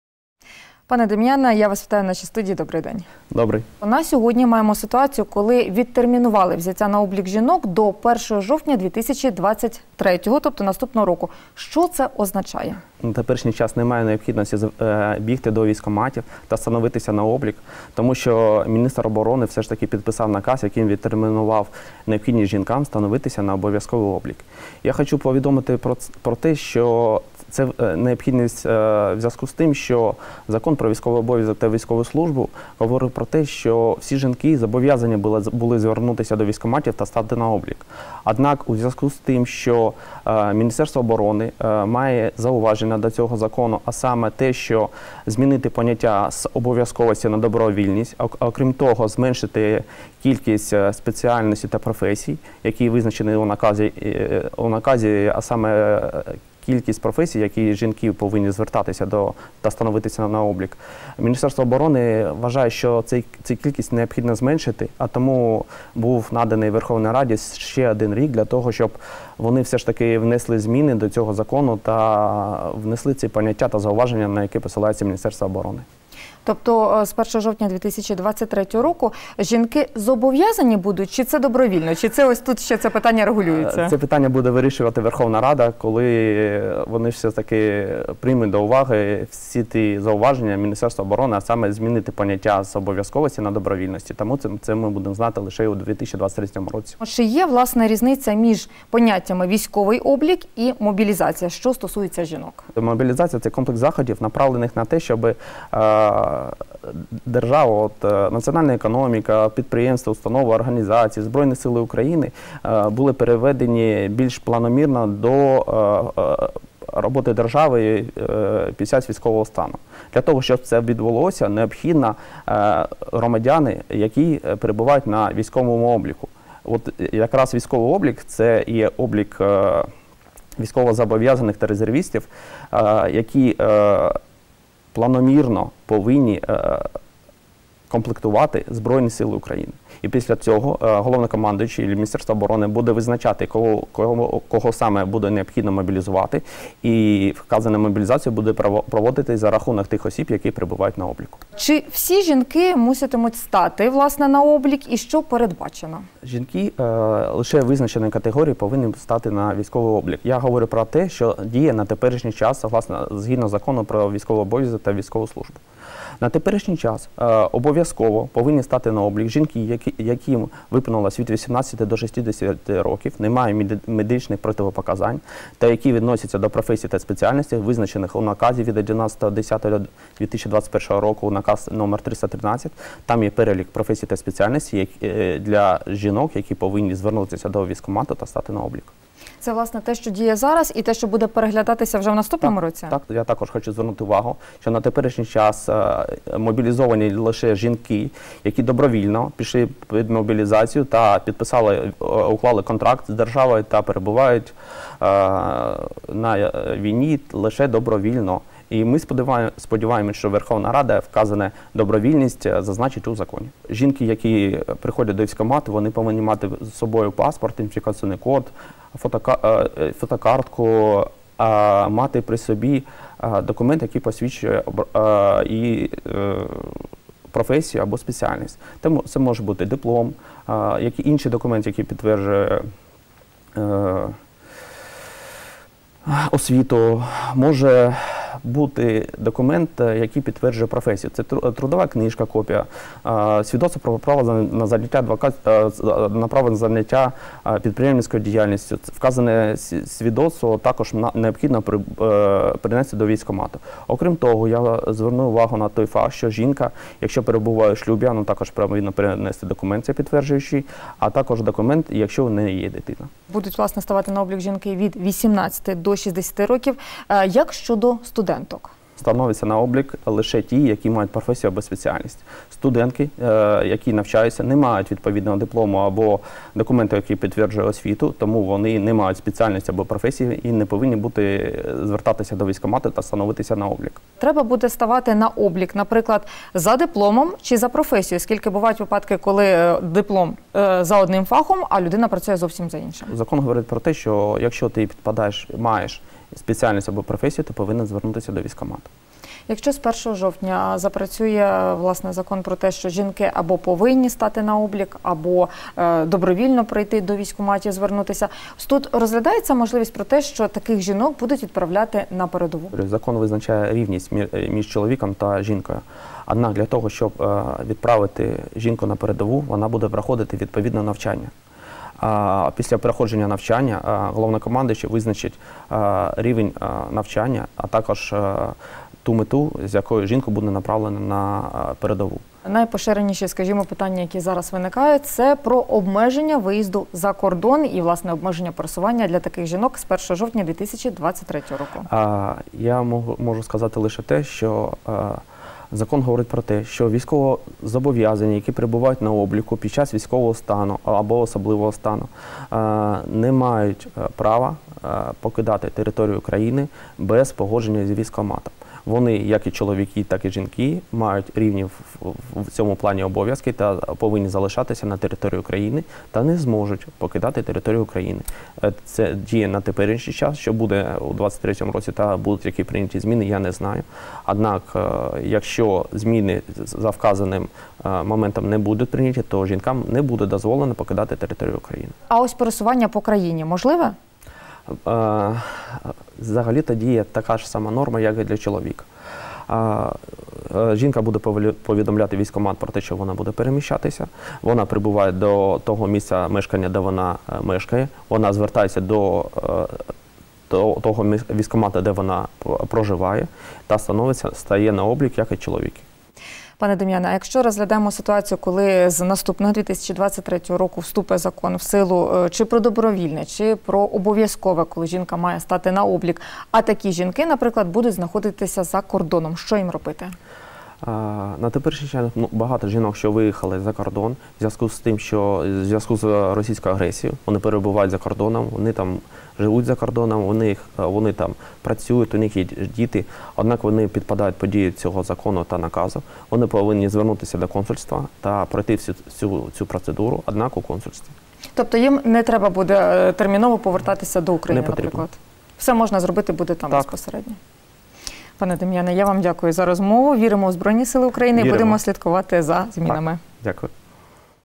Адвокат розповів про те, чи бути мобілізації жінок та хто має стати на облік | Прифронтове інтерв'ю
Що таке військовий облік для жінок, та чи означатиме це мобілізацію? Чи можна буде виїжджати за кордон жінкам з 1-го жовтня 2023 року, якщо жінка перебуватиме на військовому обліку, та чому Закон зараз знаходиться на доопрацюванні - про це та інше адвокат Дем'ян Амельченко розповів в інтерв'ю Маргариті Галіч.